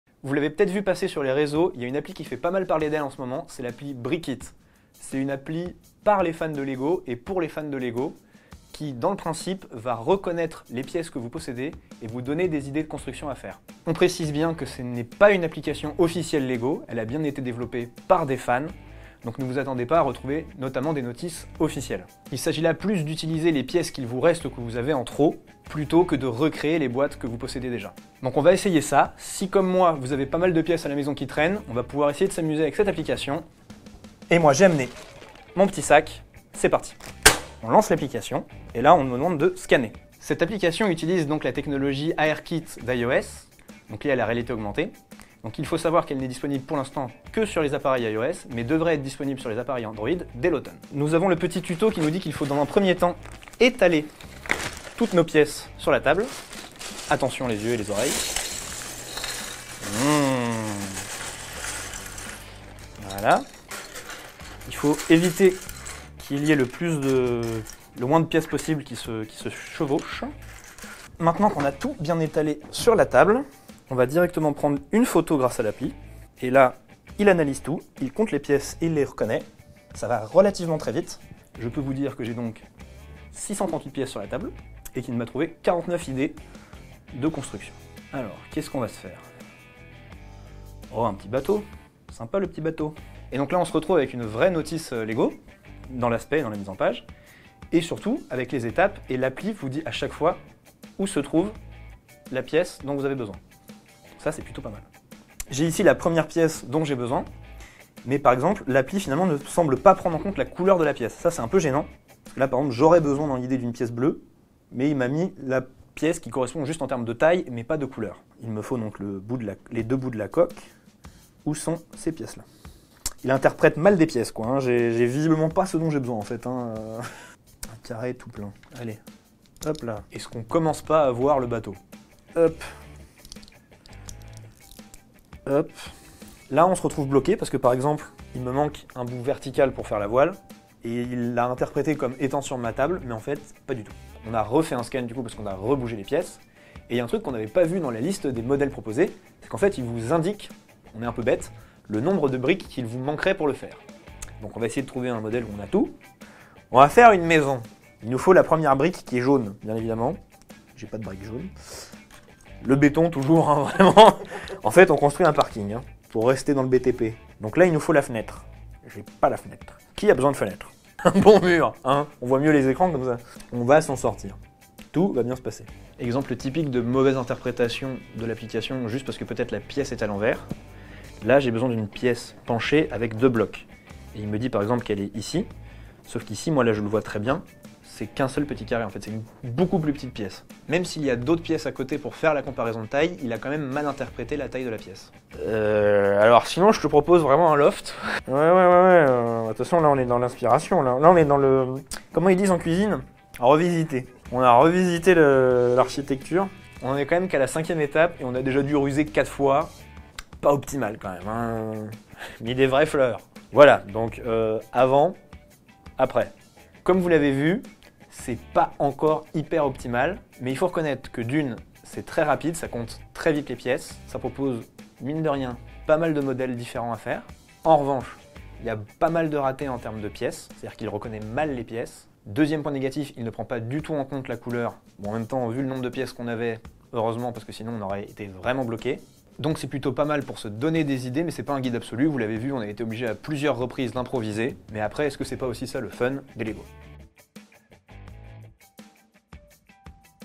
On a essayé BrickIt, l’application qui donne une seconde vie à vos Lego
01net.com est le premier site web français d’information spécialisé dans les nouvelles technologies et l’actualité des produits high-tech.